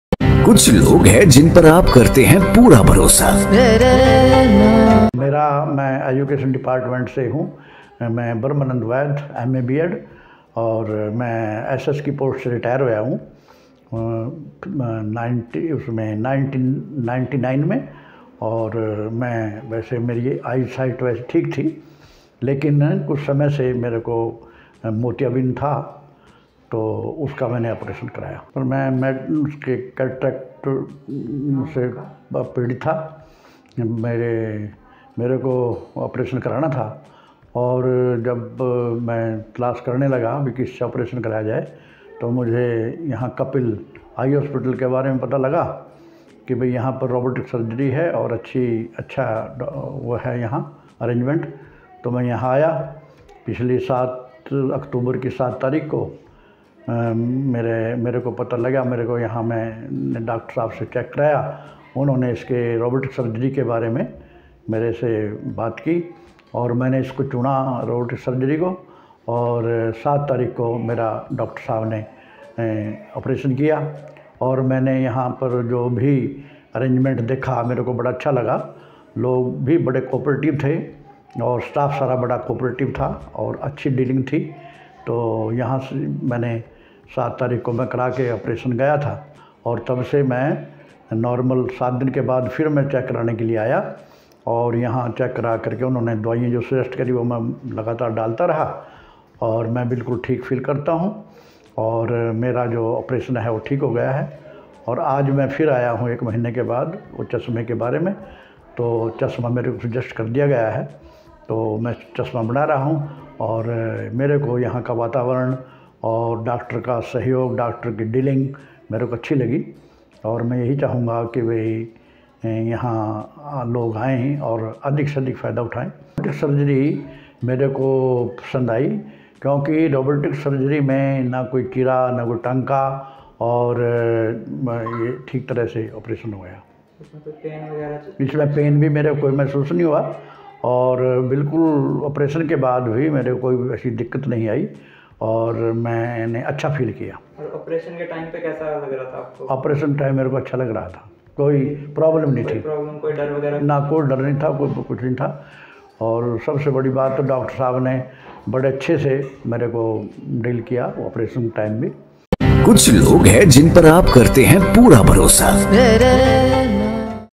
"Seeing Clearly Again: Patient's Robotic Cataract Surgery Journey | सफेद मोतियाबिंद
Femto laser cataract surgery testimonial.
Femto laser cataract surgery is the latest technique for cataract treatment. Patient can resume routine life from the next day
Follow Us on:
📍Address: Kapil Eye Hospital, 240 Vivek Vihar, Near Police Line, Ambala City, Haryana